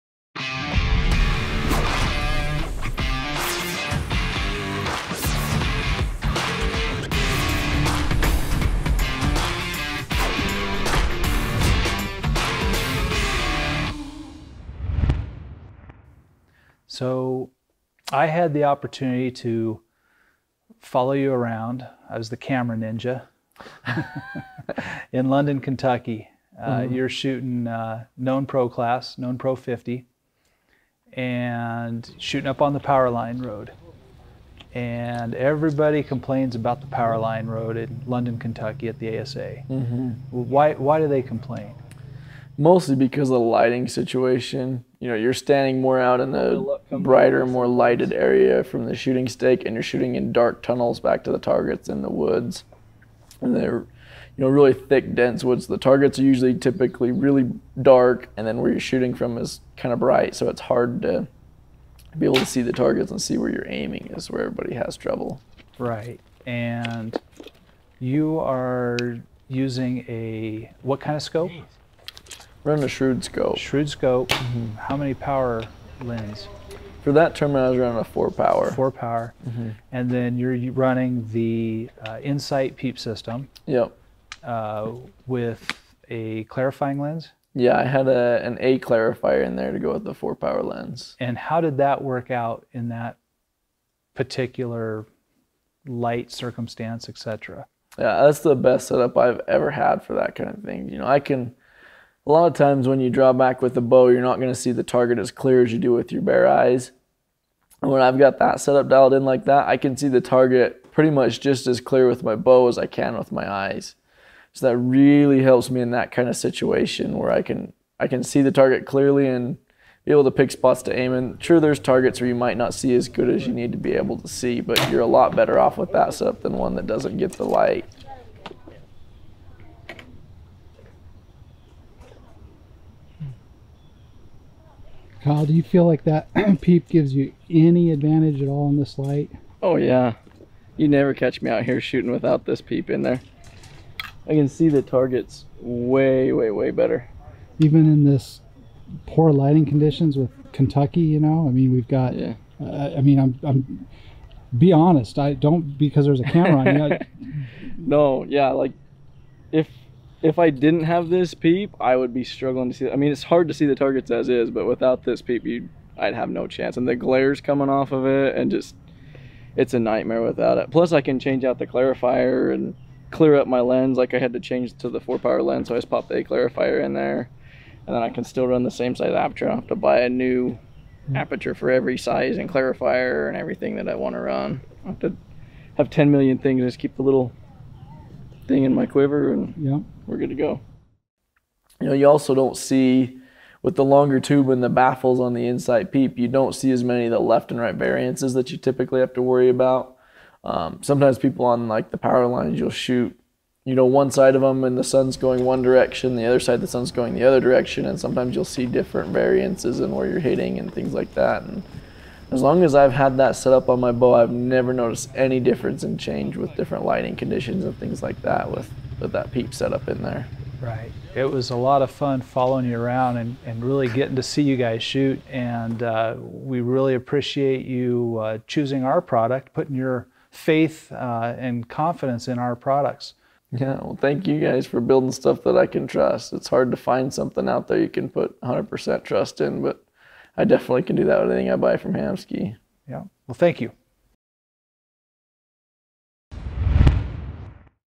Hamskea- InSight Peep Interview with Kyle Douglas
InSight Peep System

Hamskea co-founder Shawn Greathouse talks with professional target archer Kyle Douglas on why he uses the InSight Peep System and how it helps him in difficult lighting situations.